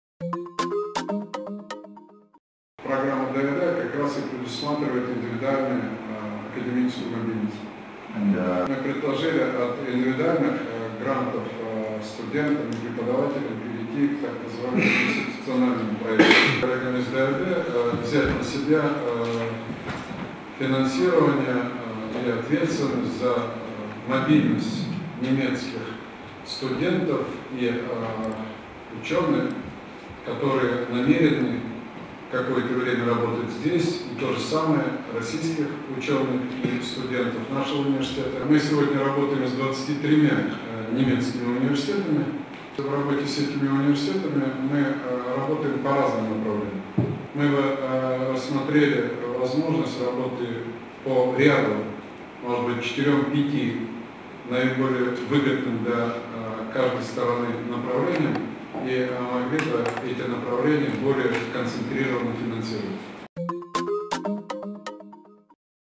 No comment. Магдебургский университет в КФУ
В Казанском федеральном университете состоялась встреча ректора КФУ Ильшата Гафурова с представителями Магдебургского университета